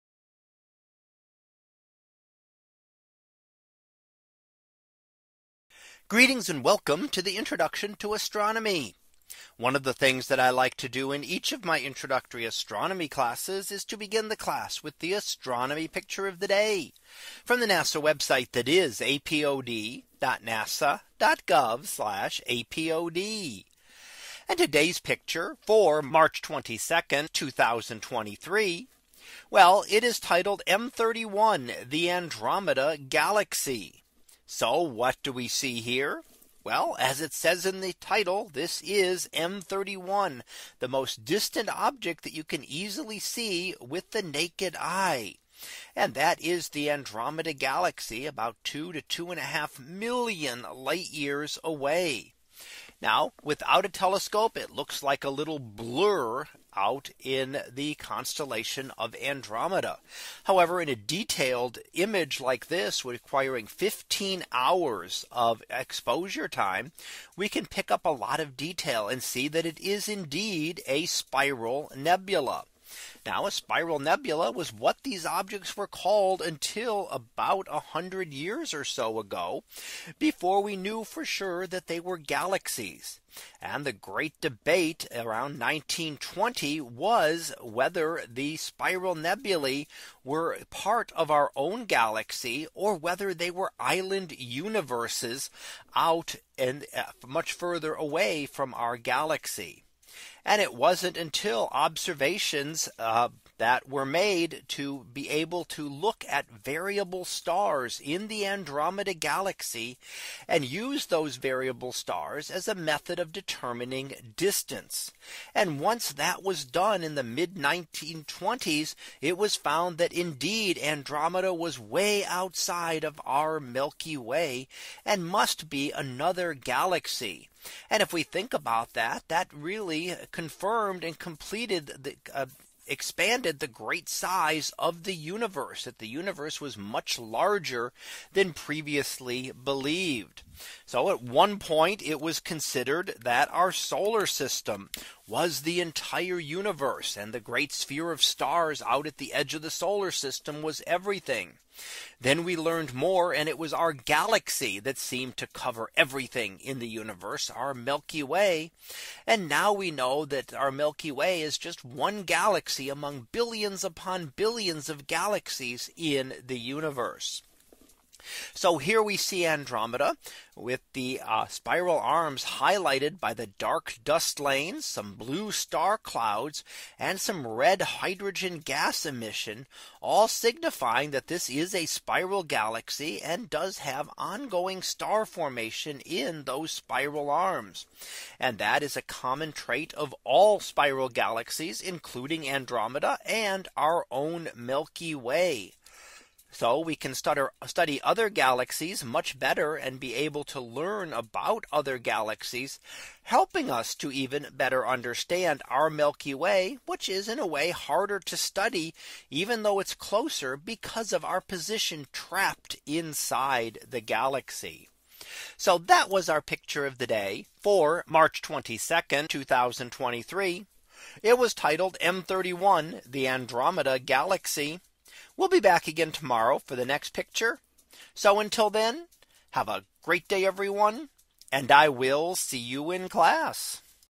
2023 March 22 - M31: The Andromeda Galaxy
In today's image, we see the Andromeda Galaxy, also known as M31. This is the nearest large spiral galaxy to our own Milky Way and is the most distant object that can be easily seen with the naked eye. Detailed studies of Andromeda and other similar galaxies can help us better understand out own Milky Way Galaxy.